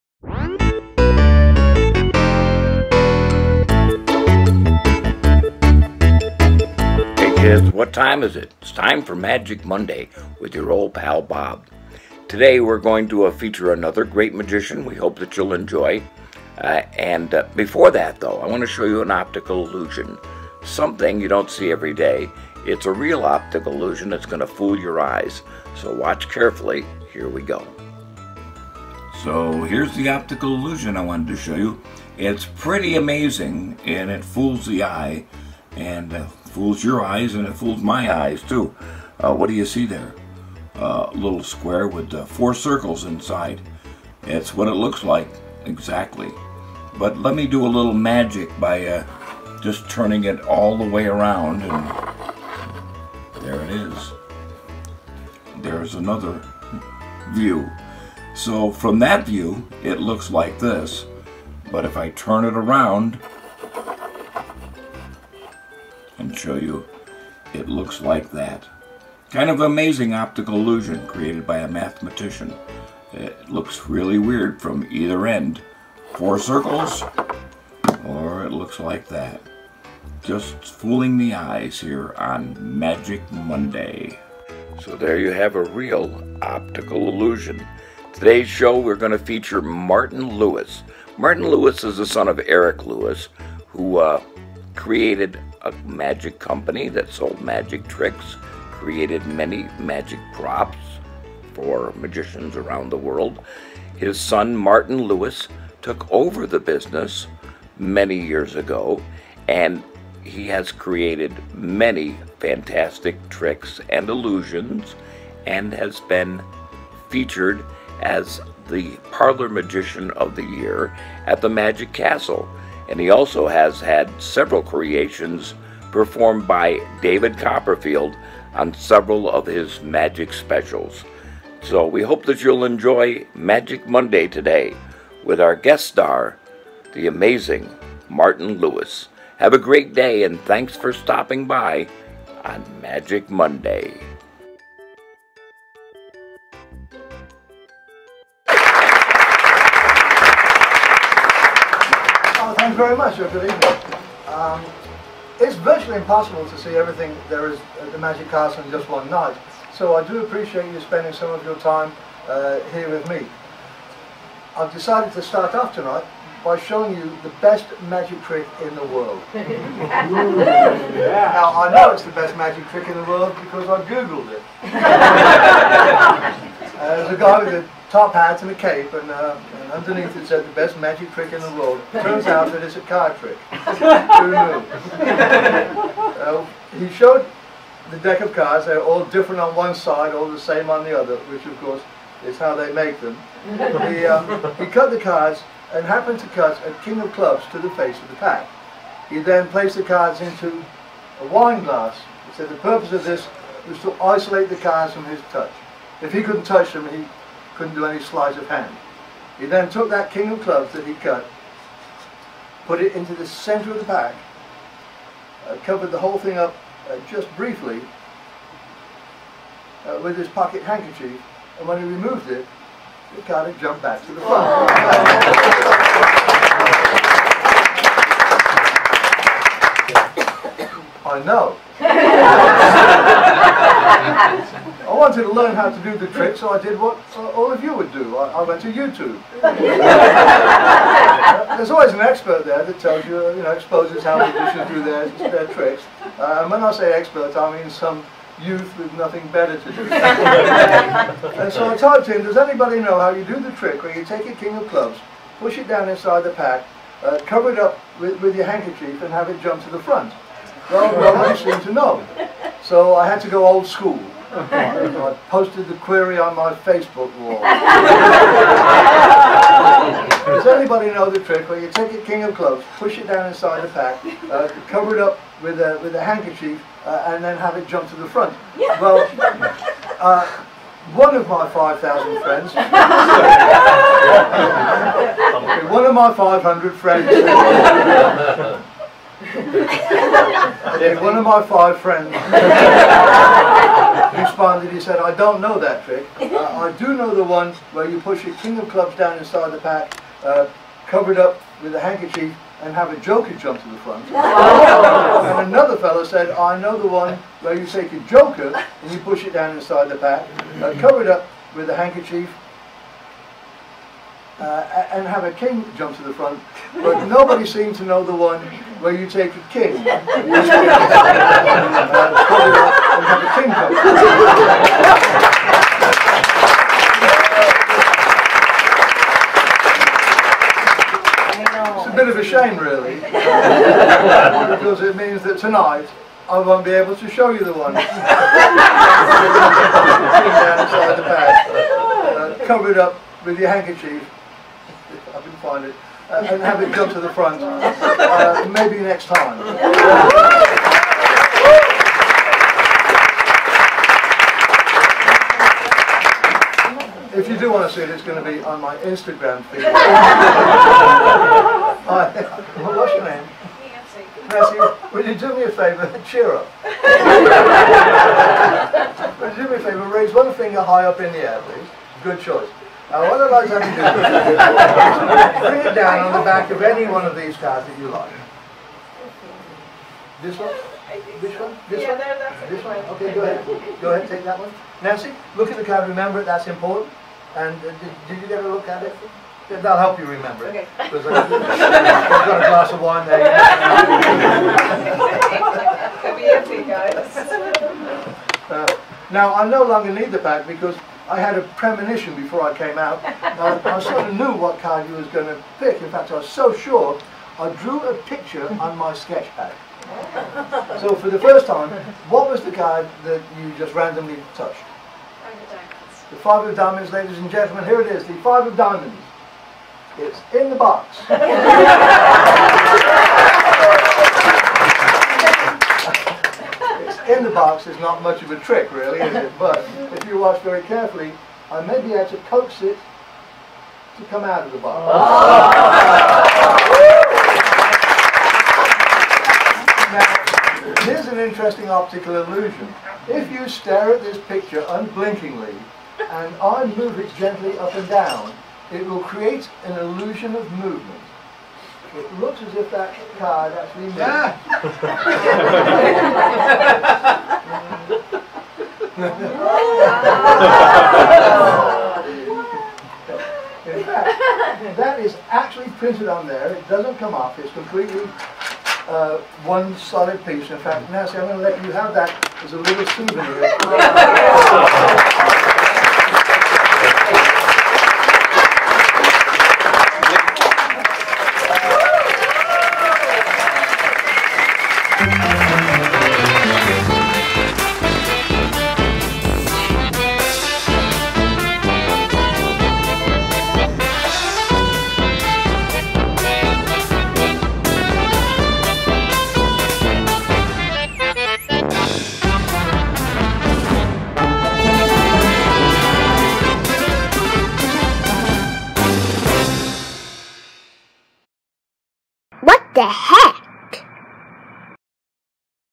Magic Monday with Martin Lewis
The Magic of Martin Lewis on Magic Monday. He is a creator of magic and has been featured at The Magic Castle. His tricks have been seen by millions on two of David Copperfield's specials. Plus, I show you an optical illusion.